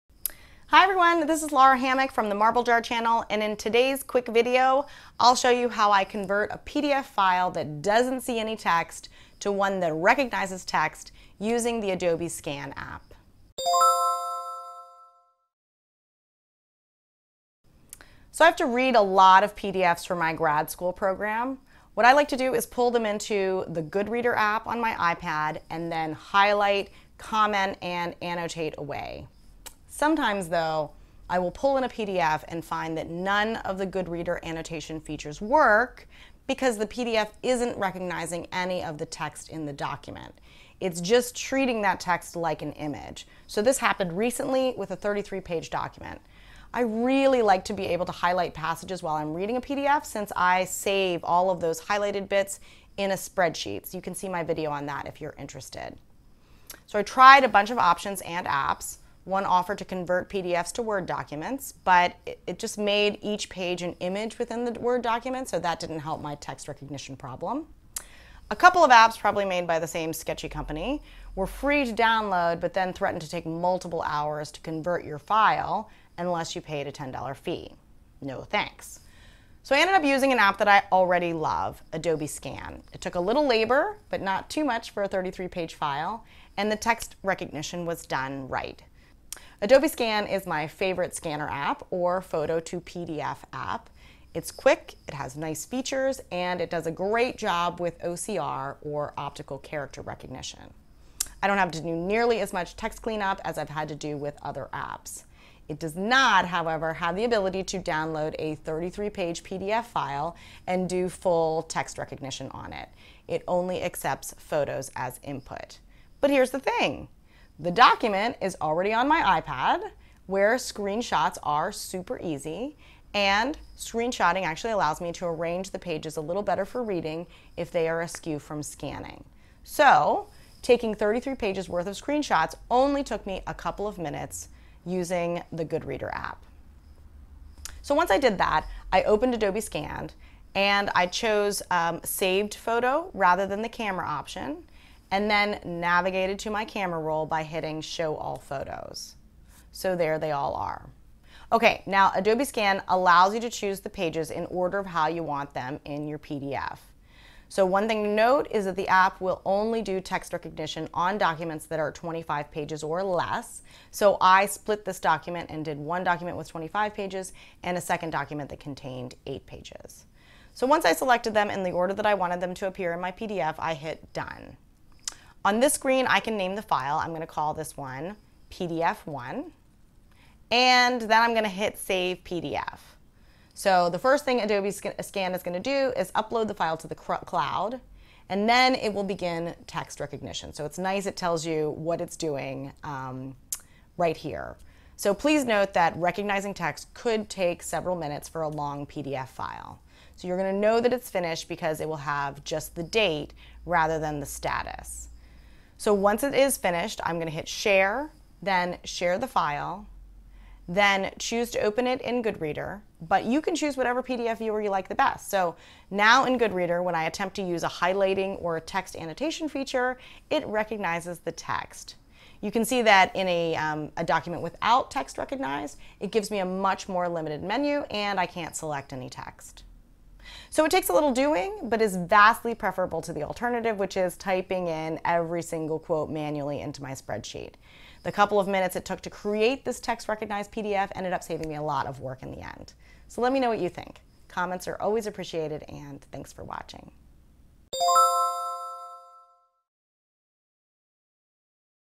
Recognize Text in Your PDF with Adobe Scan
In this video, I’ll show you how I convert a PDF file that doesn’t see any text, to one that recognizes text  using the Adobe Scan app.

*** A full transcript follows.

Hi, everyone!  This is Lara Hammock from the Marble Jar channel and in today’s quick video, I’ll show you how I convert a PDF file that doesn’t see any text, to one that recognizes text  using the Adobe Scan app.

I have to read a lot of PDFs for my graduate school program. I like to pull them into the Goodreader app on my iPad and then highlight, comment, and annotate away.  Sometimes though, I will pull in a PDF and find that none of the GoodReader annotation features work because the PDF isn’t recognizing any of the text in the document. It’s just treating that text like an image. This happened recently with a 33 page document.  I really like to be able to highlight passages while I’m reading a PDF, since I save all of the highlighted bits in a spreadsheet — you can see my video on that if you are interested.

I tried a bunch of options and apps — one offered to convert PDFs to Word documents, but it just made each page an image within the Word document, so that didn’t help my text recognition problem.  A couple of apps, probably made by the same sketchy company, were free to download, but then threatened to take multiple hours to convert your file unless you paid a $10 fee. No thanks.  I ended up using an app that I already love, Adobe Scan. It took a little labor, but not too much for a 33 page file, and the text recognition was done right.

Adobe Scan is my favorite scanner app or photo to PDF app. It’s quick, has nice features, and does a great job with OCR or optical character recognition.  I don’t have to do nearly as much text clean up as I’ve had to do with other apps. It doesn’t not, however, have the ability to download a 33-page PDF file and do full text recognition on it.  It only accepts photos as input.  But here is the thing — the document is on my iPad where screen shots are super easy AND screen shotting allows me to arrange the pages a little better for reading if they are askew from scanning.  Taking 33 pages worth of screen shots only took me a couple of minutes within the GoodReader app.

Once I did that, I opened Adobe Scan, chose the Saved Photo rather than the Camera option and navigated to my Camera Roll by hitting Show All Photos. There they all are. Okay — now Adobe Scan lets you chose the pages in order of how you want them in your PDF. One thing to note is that the app will only do text recognition on documents that are 25 pages or less, so I did one document with 25 pages and a second document that contained 8 pages.

Once I selected them in the order I want them to appear in my PDF, I hit Done. On this screen I can name the file — I’ll call this one “PDF 1” and hit Save PDF.  First Adobe Scan will upload the file to the cloud, then it will begin text recognition. It’s nice that is tells you what it is doing — please note that recognizing text could take several minutes for a long PDF file. You’ll know that it is finished because it will just have the date rather than the status. Once it is finished, I hit Share, then Share File, then choose to open it in GoodReader, but you can choose whichever PDF viewer you like best.  So, now in GoodReader, when I attempt to use a highlighting or other text annotation feature, it recognizes the text.  You can see that in a document without text recognized, it gives me a much more limited menu and I can’t select any text.

So, it takes a little doing, but is vastly preferable to the alternative, which is typing in each quote manually into my spreadsheet. The couple of minutes it took to create this text recognized PDF ended up saving me a lot of work in the end. Let me know what you think!